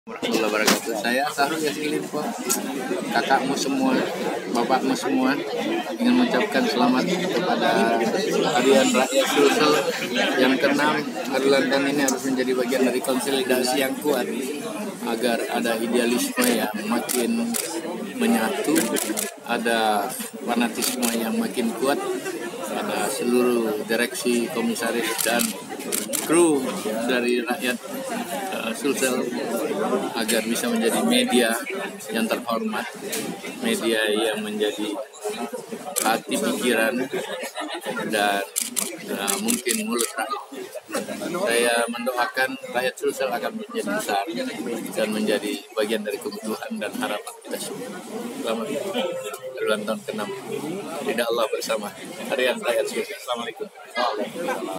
Ucapan HUT RAKSUL ke-6 oleh Syahrul Yasin Limpo
Mantan Gubernur Sulsel dua periode, Syahul Yasin Limpo tidak ingin ketinggalan mengucapkan HUT Raksul ke-6.